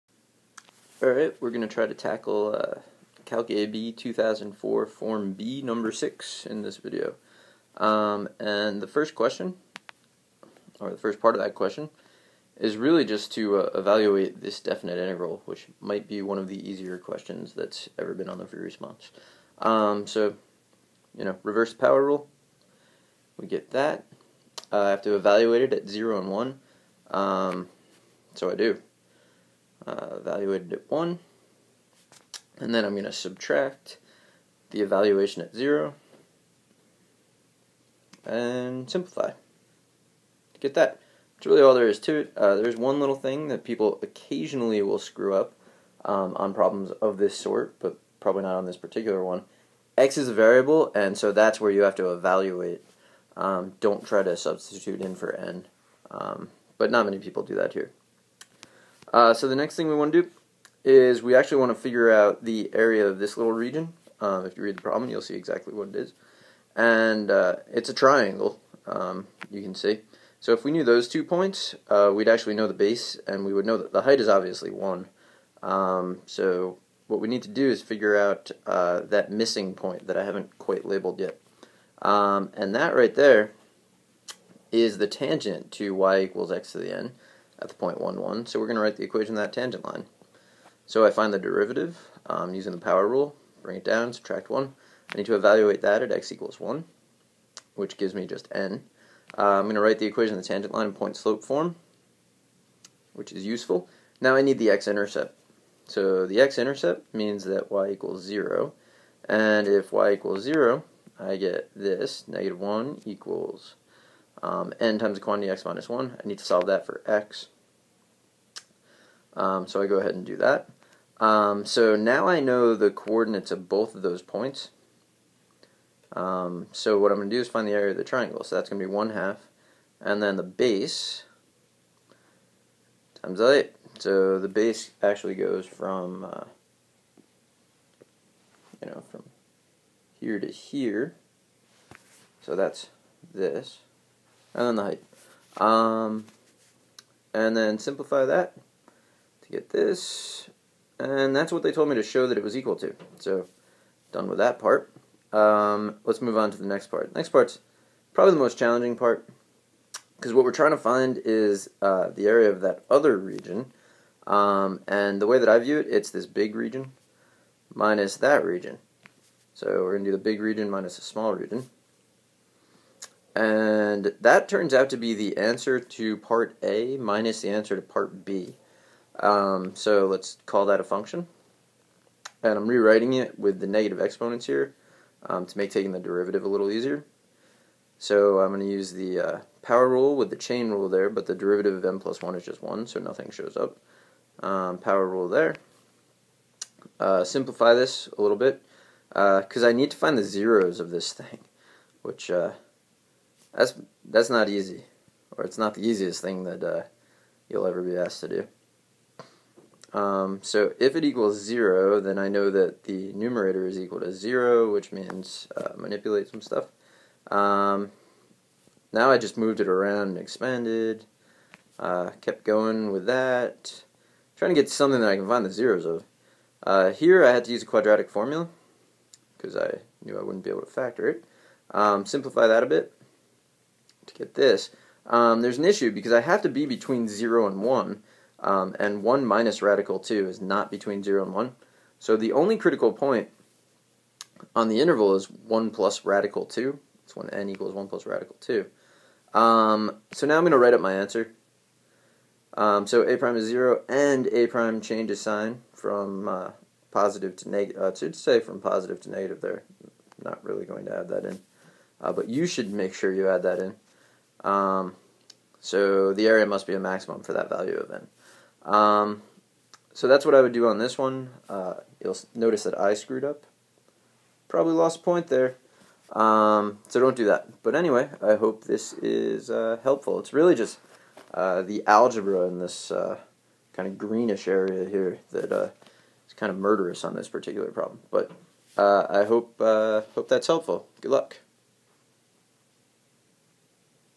Calc AB & Calc BC 2004 (Form B) FRQ #6
2004 AP Calculus AB & Calculus BC Form B number 6.

You'll notice I forget to say how the sign of the derivative changed at the critical point that I found...don't do that!

Topics: definite integral, tangnet line, optimization